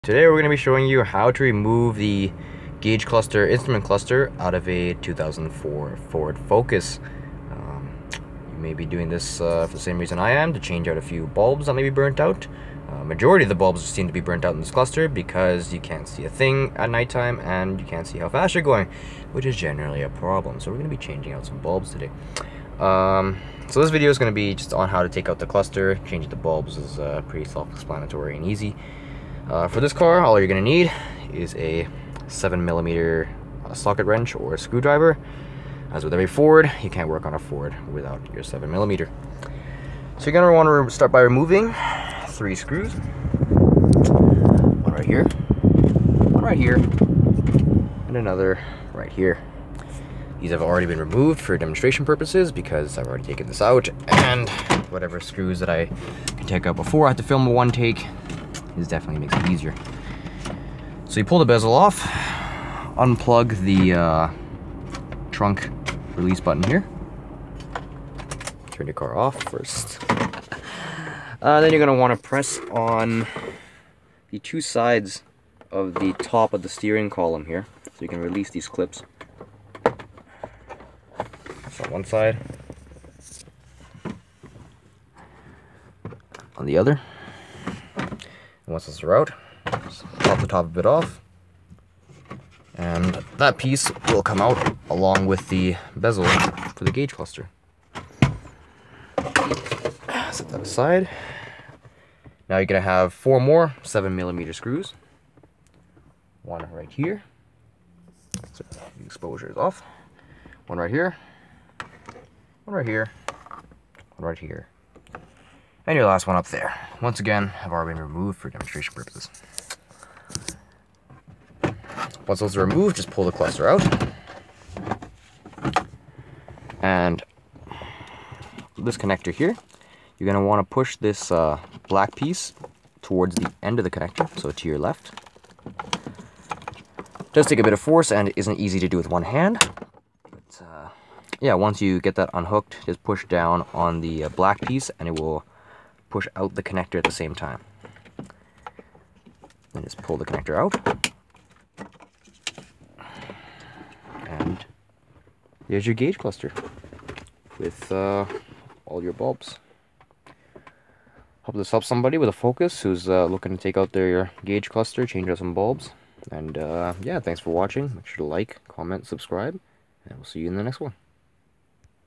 Ford Focus instrument cluster removal
Another one-take, this time on how to remove the gauge cluster on a first generation (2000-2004) Ford Focus. Should apply to both sedan and hatchback models. Disconnecting your battery prior to this procedure is recommended. Reassembly is in simply the reverse of the removal process.

A pair of small pliers would help in removing and installing bulbs, as they seem to get screwed in there pretty tight.